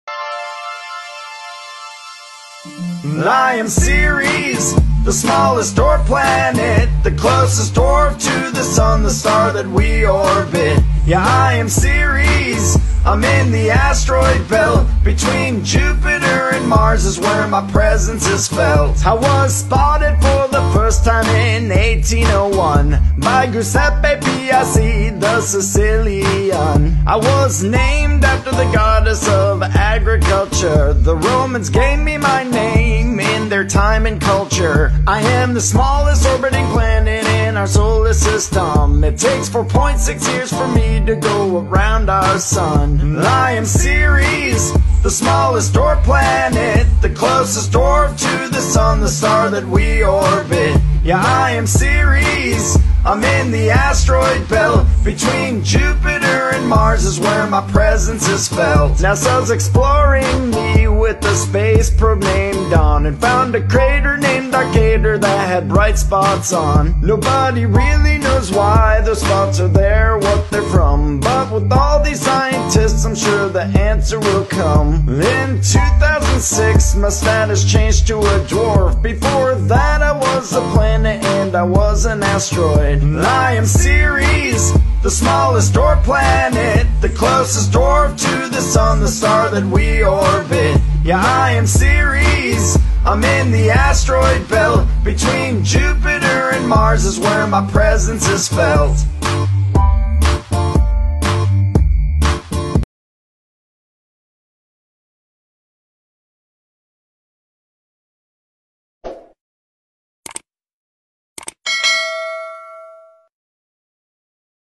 Ceres Dwarf planet
Ceres was the first object discovered in the main asteroid belt and is named for the Roman goddess of agriculture. Italian astronomer Father Giuseppe Piazzi spotted the object in 1801. Ceres was initially classified as a planet and later classified as an asteroid as more objects were found in the same region. (Images credit goes by Google)